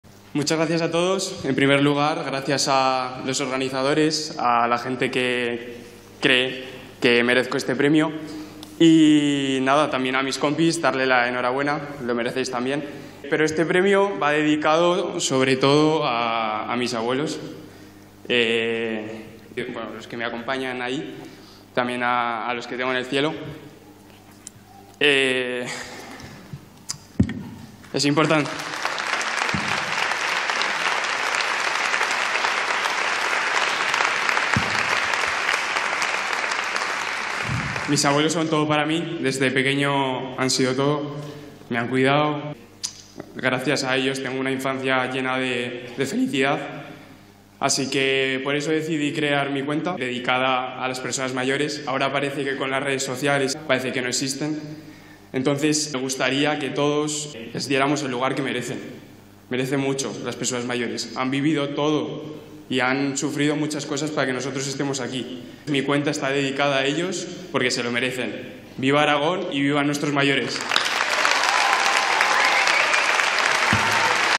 ¡Me dan el PREMIO a MEJOR CREADOR REVELACIÓN 2024!
•𝐆𝐑𝐀𝐂𝐈𝐀𝐒 𝐀 𝐌𝐈𝐒 𝐀𝐁𝐔𝐄𝐋𝐎𝐒•
Este fue mi discurso en la gala “La Noche de la Influencia” de Aragón Influye donde conseguí el Premio a Mejor Creador Revelación 2024.
Aún sigo asimilando todo lo que viví.
No solo por el premio sino porque la gala fue ESPECTACULAR. Conocí a gente INCREÍBLE con proyectos MUY, MUY VALIOSOS.
Y yo… me quedo con eso. Me quedo con que hay gente bonita que a pesar de las dificultades sabe crear contenido de CALIDAD. Personas que apuestan por la creatividad y la positividad. Personas que hacen de este mundo un lugar mejor.
Muy orgulloso, de verdad.
Gracias a todos por formar parte de esta comunidad tan mágica. A mi familia, por apoyarme.
Y como no, gracias a mis abuelos, que son todo para mi.